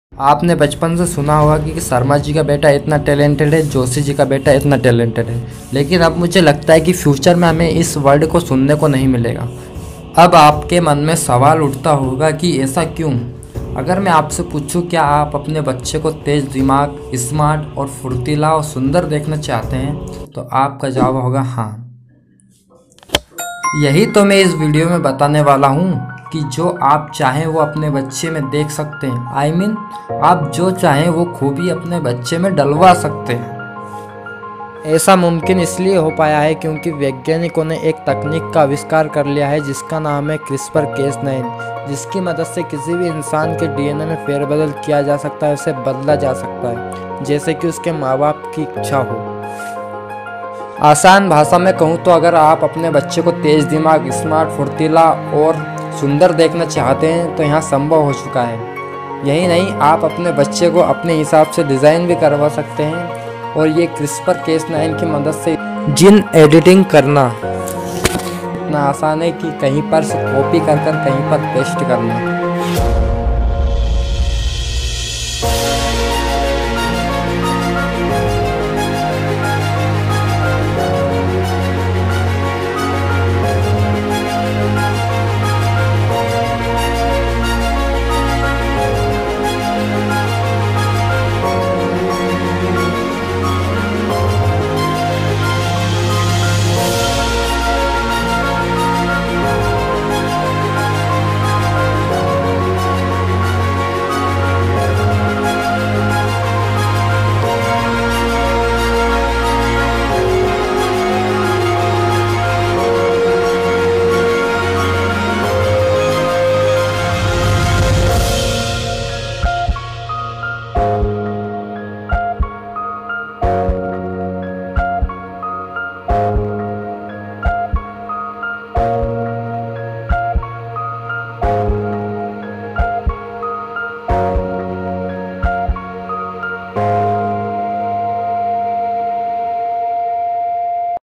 इस तकनीक से हम मन चाहे इंसान बना सकते हैं| gene editing crispr cas9 in Hindi. Sahilistic!
hello doston is video mein  Gene editing ke bare mein bataya hai  jo ki crispr cas9 se aasani se ki Ja sakti hai